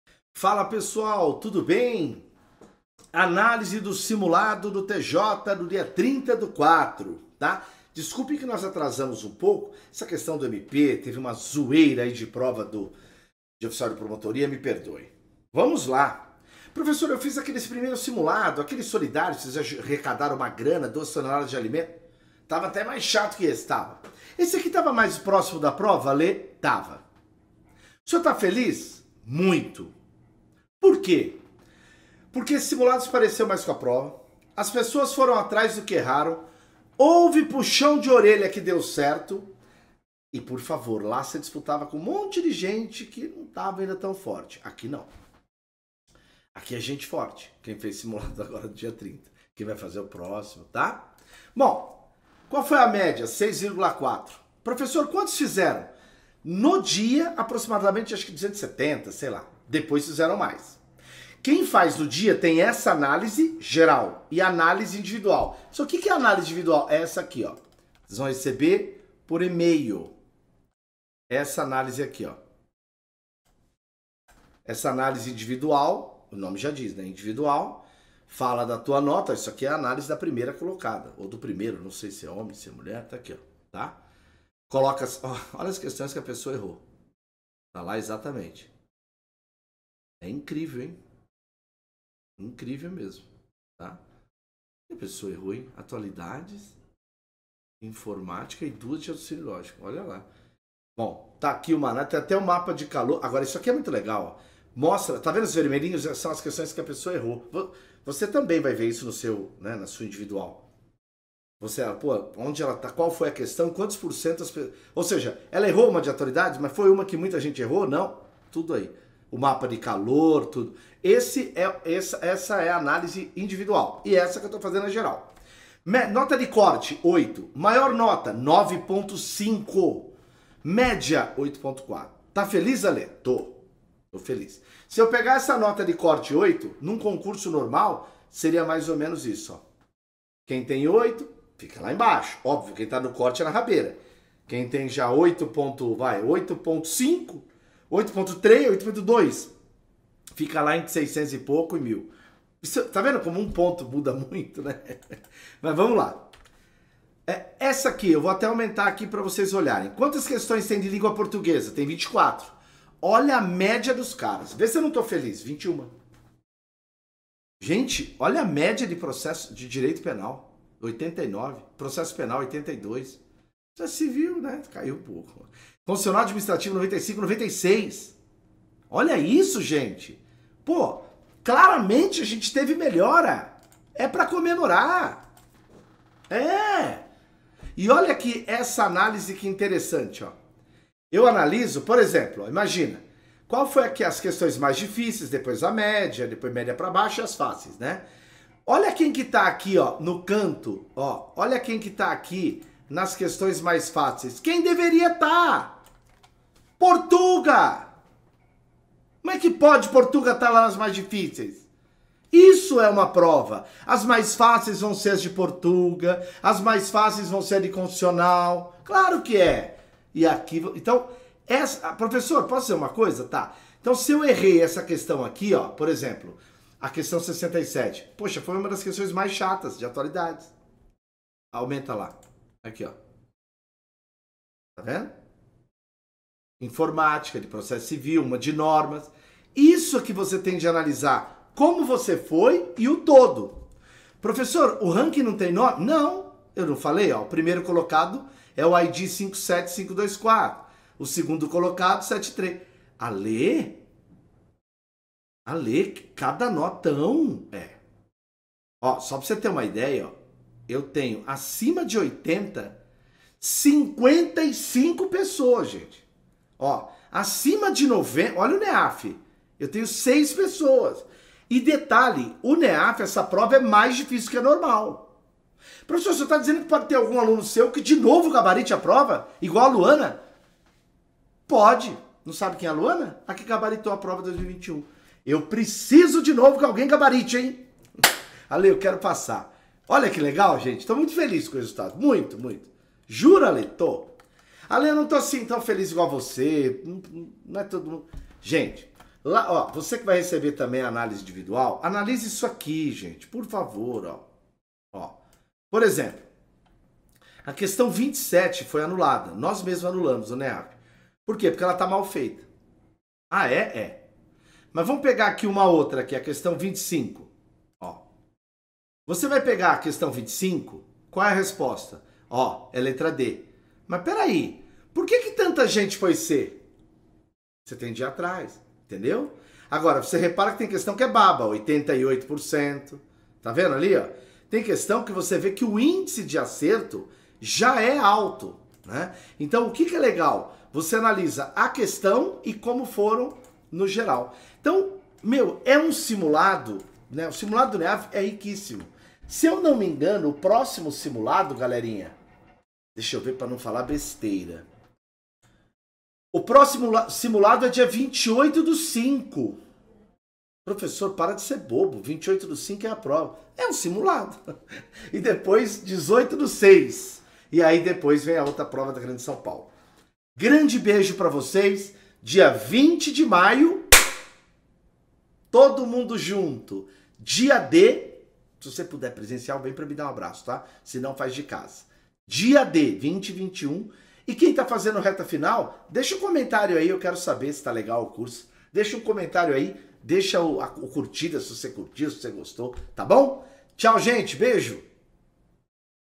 Análise Global do Simulado 1 Escrevente TJSP 2023 - 30/04
Com a prova de Escrevente Técnico Judiciário cada dia mais perto, uma das melhores maneiras de revisar o conteúdo estudado é por meio de simulados.

O NEAF Simulou o dia da prova de Escrevente TJSP mais uma vez! O professor Alessandro Ferraz fala sobre o desempenho geral de todos os alunos que fizeram o simuladão. Como você acha que foi o desempenho dos alunos do NEAF?

Concurso público é aqui, no NEAF é claro!

CESPE, VUNESP, FCC, FGV e CESGRANRIO
Dúvidas, sugestões ou para mais informações: